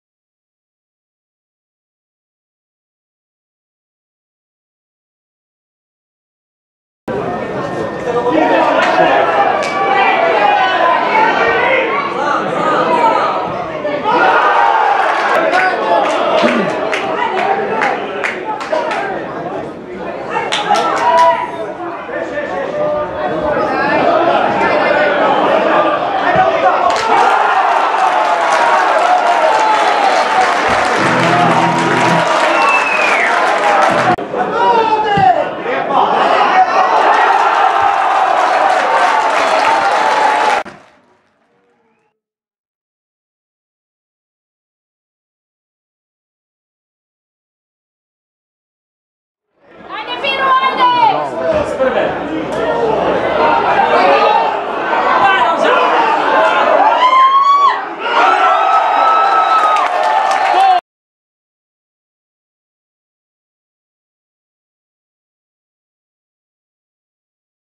Odranci - Nafta 1903 2:4
Zadetki s tekme Odranci - Nafta 1903 v 19. krogu 3. SNL-vzhod